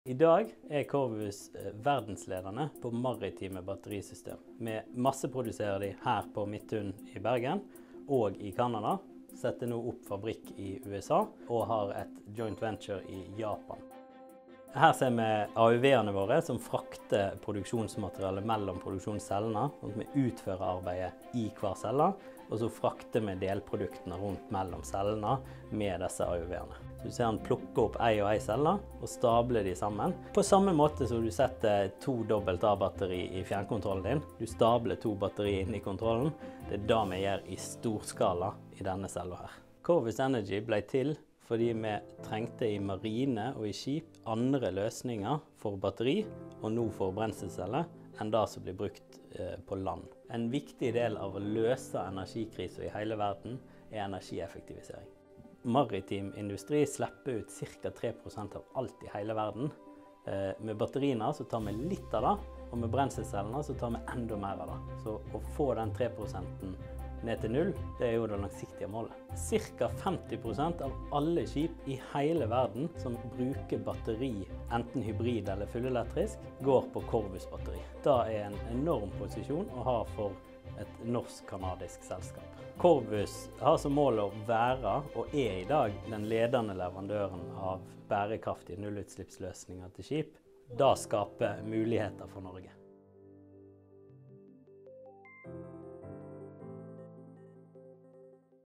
Corvus Energy er verdensledende på batterier for maritim næring
Maritim industri slipper ut rundt 3 % av utslippene i hele verden. Corvus Corvus Energy utvikler batterier og brenselceller av hydrogen til skip, med det langsiktige målet å få utslippene fra skipsfarten ned til null. De var med oss på Innovasjonstalen 2022.

Innovasjon Norge

innovasjonnorge.no